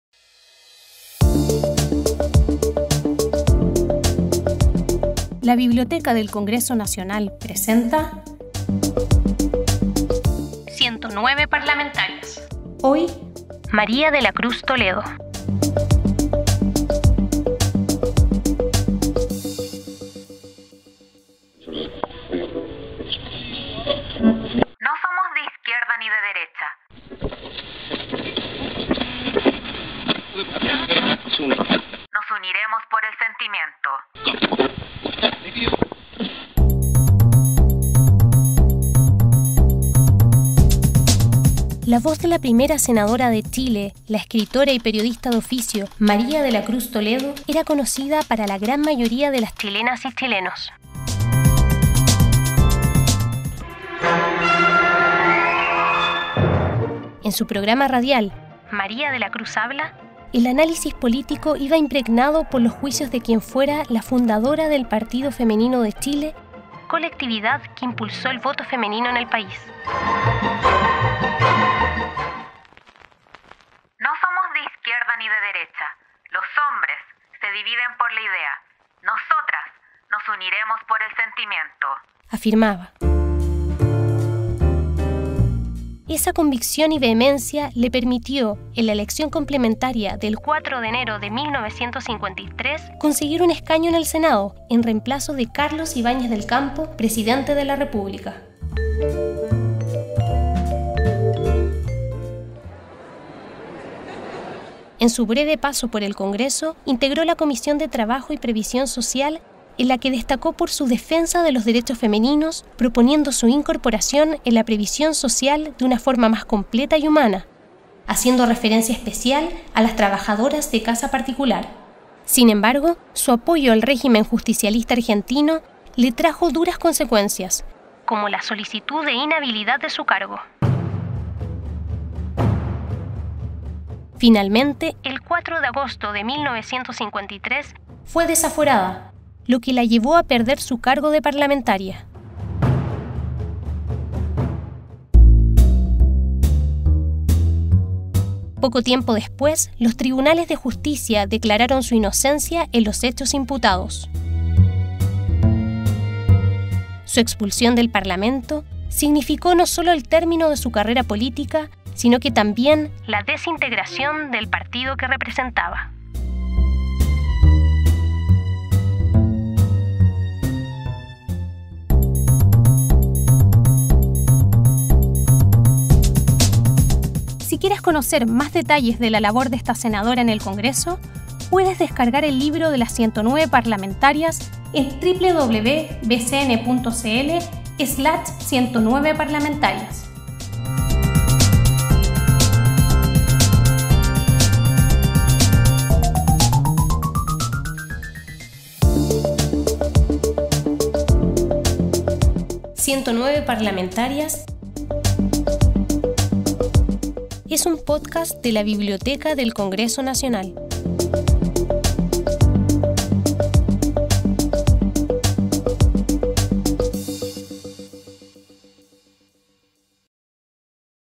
SERIE PODCAST BCN: MUJERES EN EL CONGRESO - MARIA DE LA CRUZ TOLEDO
Material elaborado en el marco de la publicación del libro "109 parlamentarias", editado por la Biblioteca del Congreso Nacional de Chile en marzo de 2020.